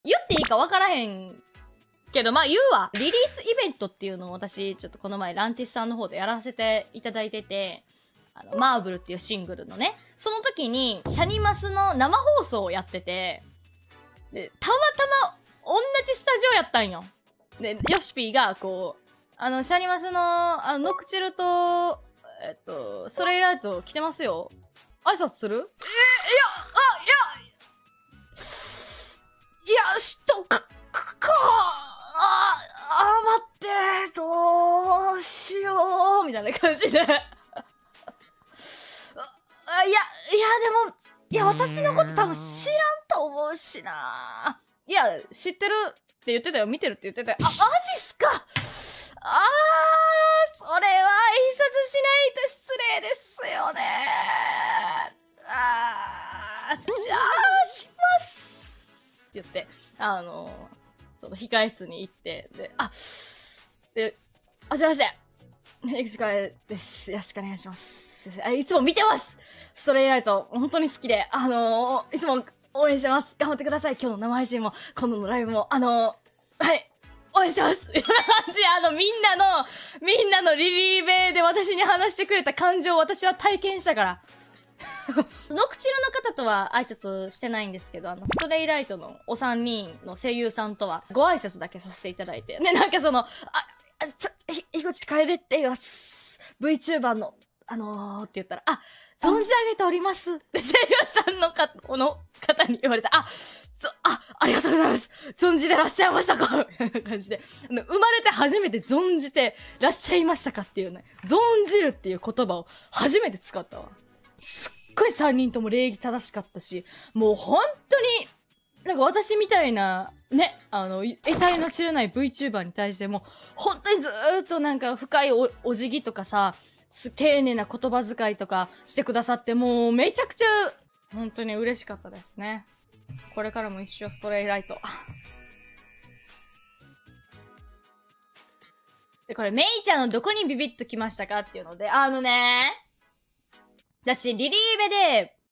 仕事先で偶然シャニマス声優と遭遇し限界化するでろーん【樋口楓/にじさんじ切り抜き】
切り抜き元
お雑談をいたします！１７【にじさんじ / 樋口楓】（切り抜きの続きから始まります）